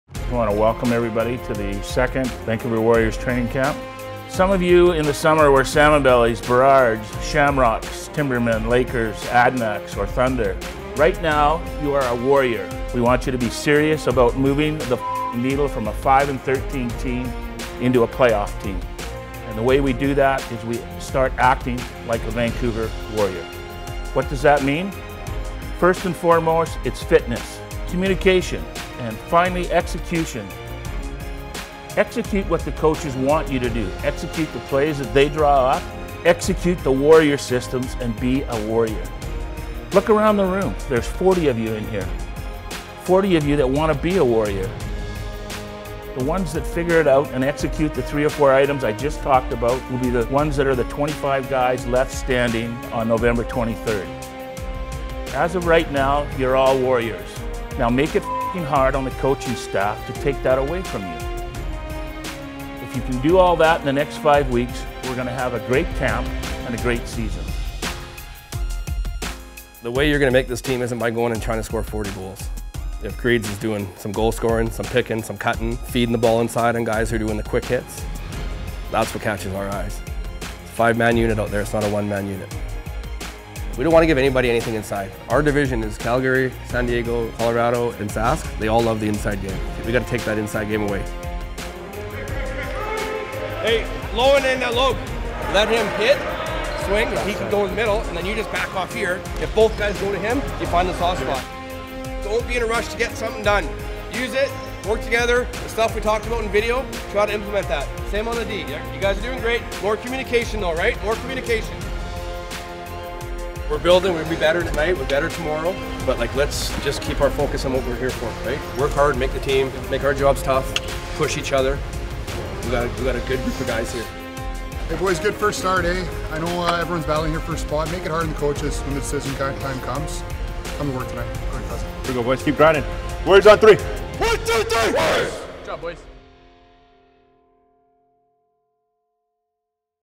Warriors Training Camp | All Access
Get an All Access, behind-the-scenes look at Warriors training camp! Listen in as GM Dan Richardson and Head Coach Chris Gill fire up the team, plus get up close to the guys on the turf as they battle for a roster spot.